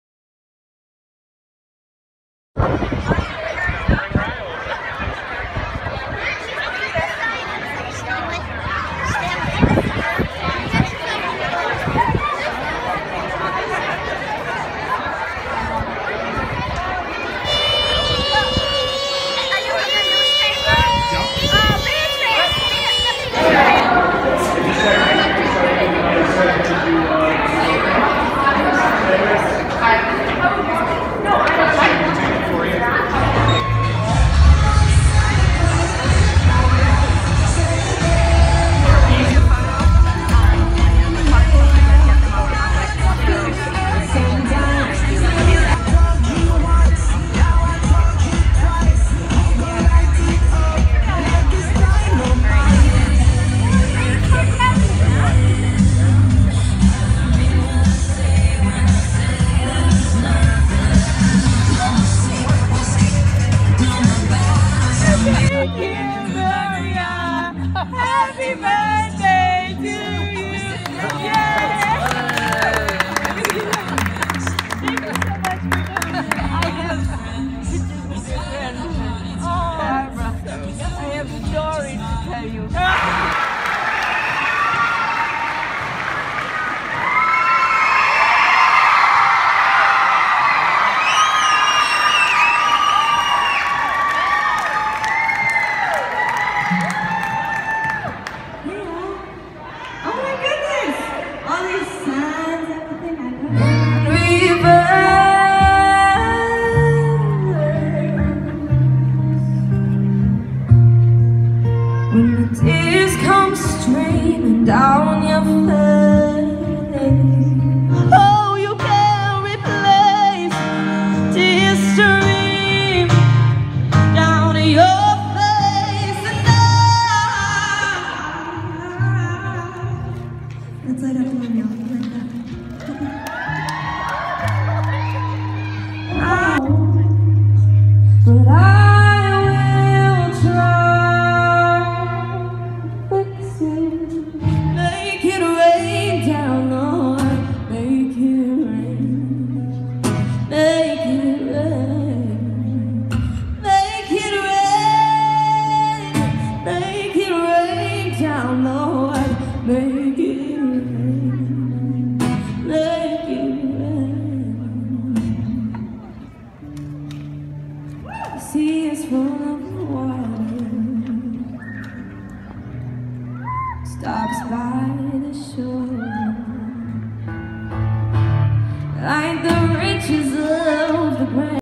Brynn Cartelli "The Voice" Homecoming Party fills MassMutual Center in Springfield
Brynn Cartelli, of Longmeadow,  Massachusetts, winner of “The Voice” Season 14, was cheered on by more than 1,200 fans at a concert at the MassMutual Center in Springfield.